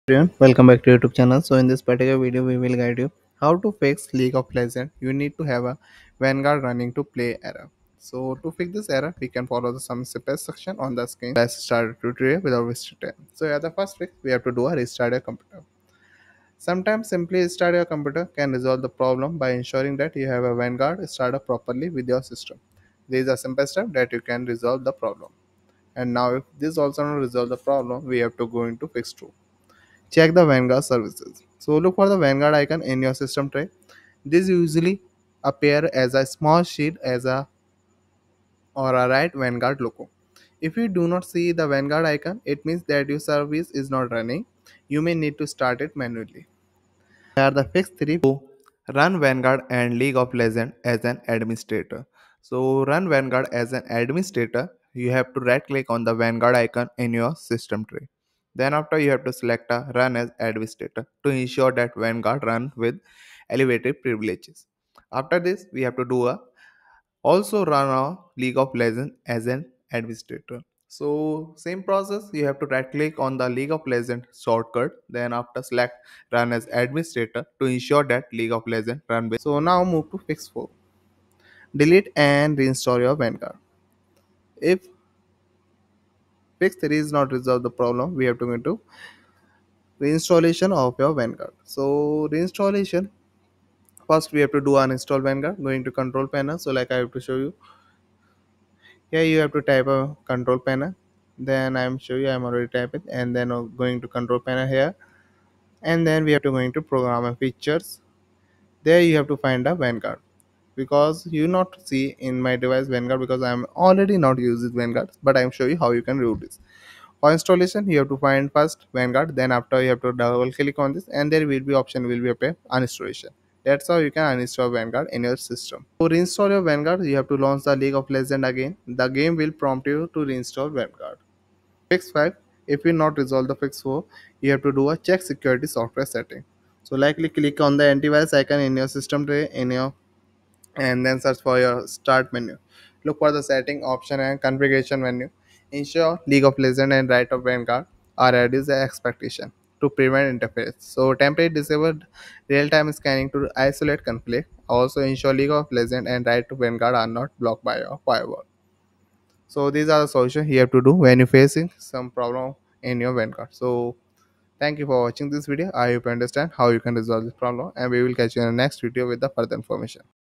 How To Fix League Of Legends “You Need To Have Vanguard Running To Play” Error
If you're encountering the frustrating "You Need To Have Vanguard Running To Play" error while trying to launch League of Legends, this YouTube video is here to help. We understand how important it is for gamers to enjoy their favorite game without any interruptions. In this tutorial, we will walk you through the process of fixing this error by ensuring that the Vanguard anti-cheat system is running correctly on your system. By following our step-by-step instructions, you'll be able to resolve the issue and get back to playing League of Legends smoothly.

00:00 Introduction
00:13 Fix 1. Restart Your Computer
00:29 Fix 2. Check The Vanguard Service
00:48 Fix 3. Run Vanguard And League Of Legends As An Administrator
01:26 Fix 4. Delete and Reinstall Vanguard
02:13 Fix 5. Check Security Software Settings